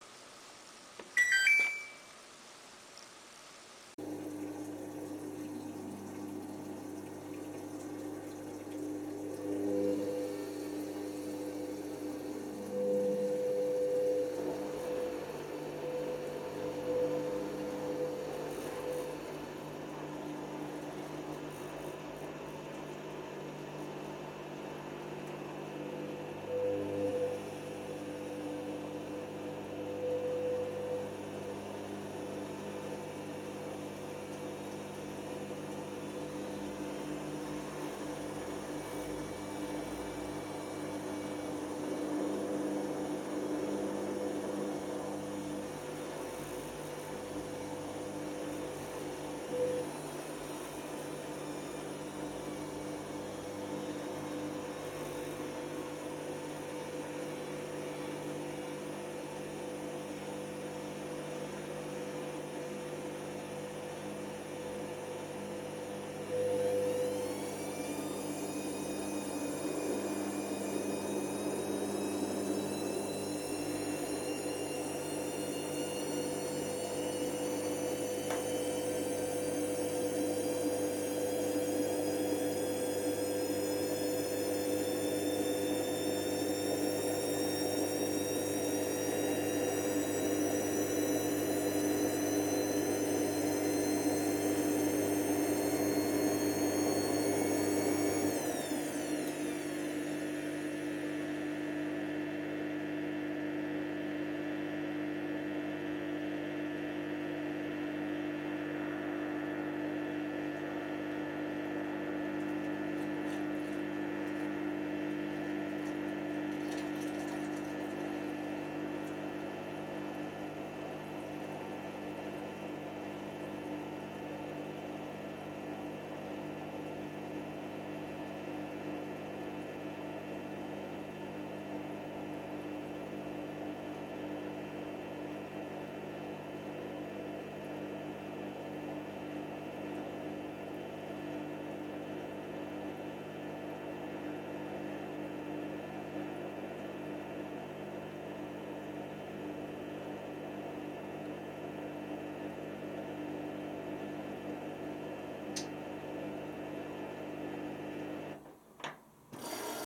Hoover Nextra HNL6166 : Duvet : Big suds lock Inter Spin (Pt 2 of 4)
washing a single duvet, first spin after the wash phase and inevitably it creates a big sudslock. it dose not complete the full inter spin.